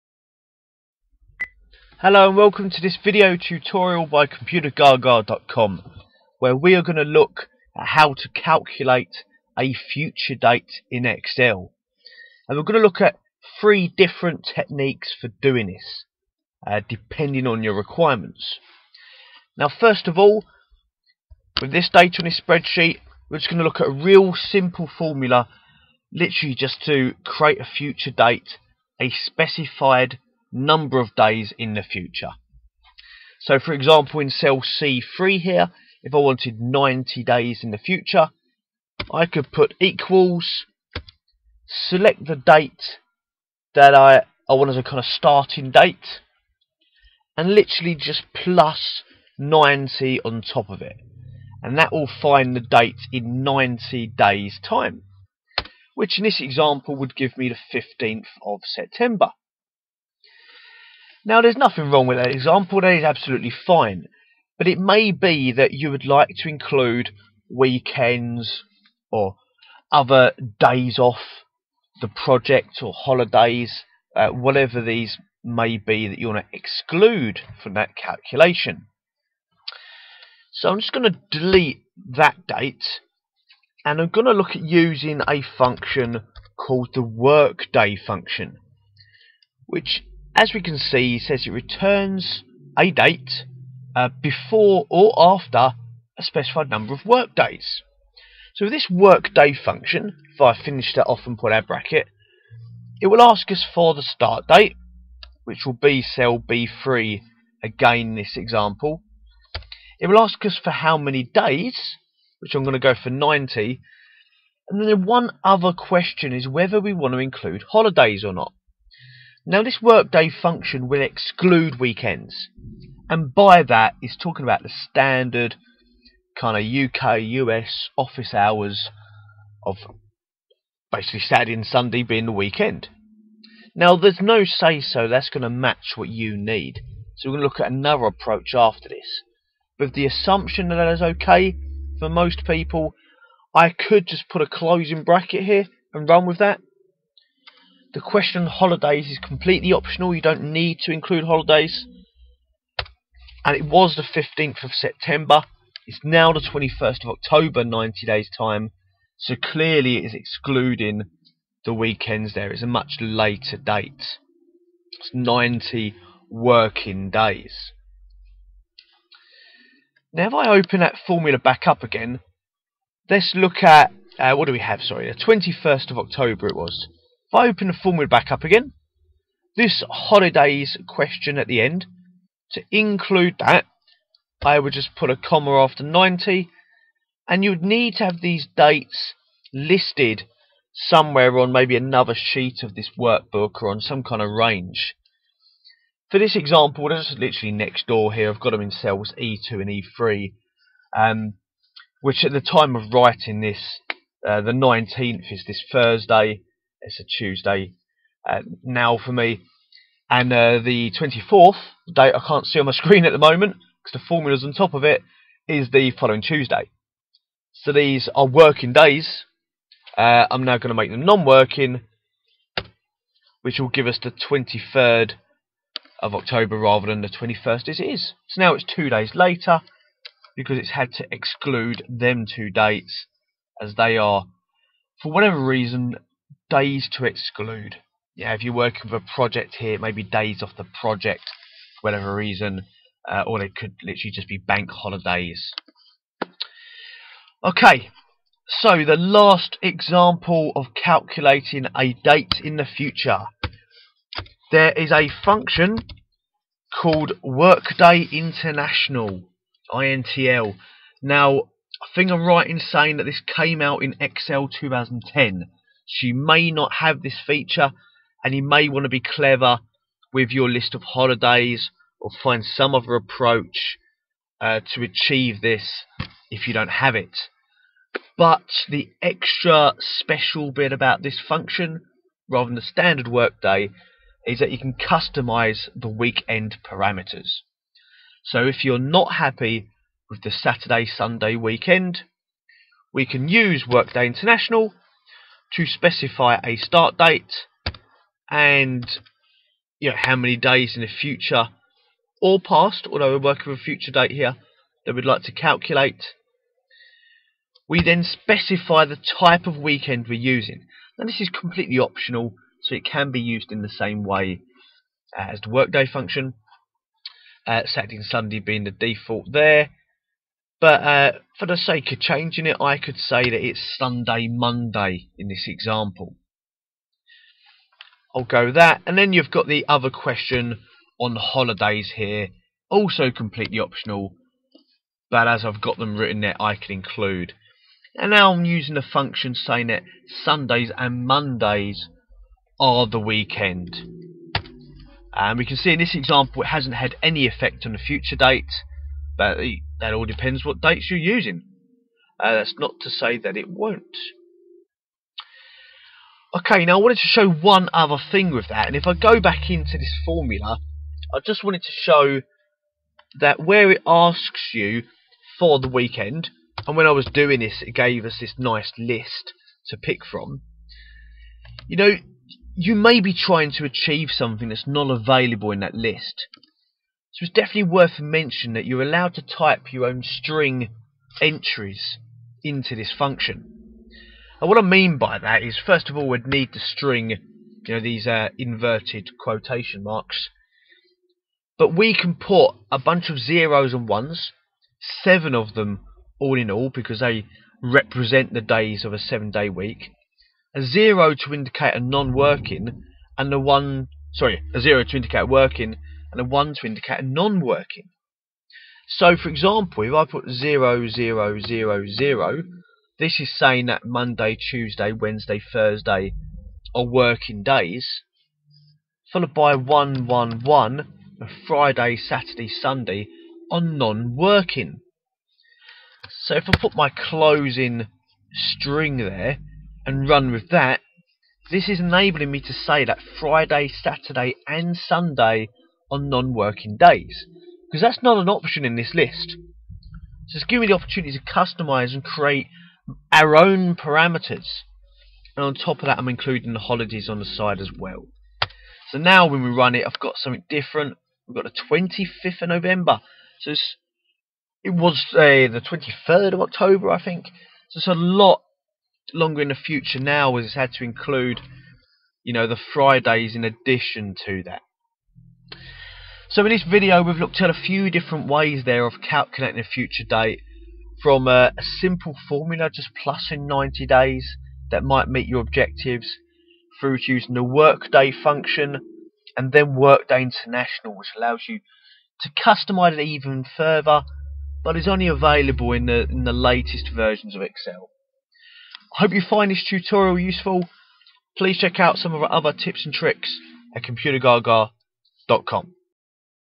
Calculate a Future Date in Excel - WORKDAY and WORKDAY.INTL Functions
Calculate a future date in Excel using formulas. This video demonstrates 3 different techniques to calculate a future date in Excel using the WORKDAY and WORKDAY.INTL functions.

The Excel WORKDAY function enables you to calculate a future or past date and will exclude weekend and specified holiday days.

Learn over 150 of the best Excel functions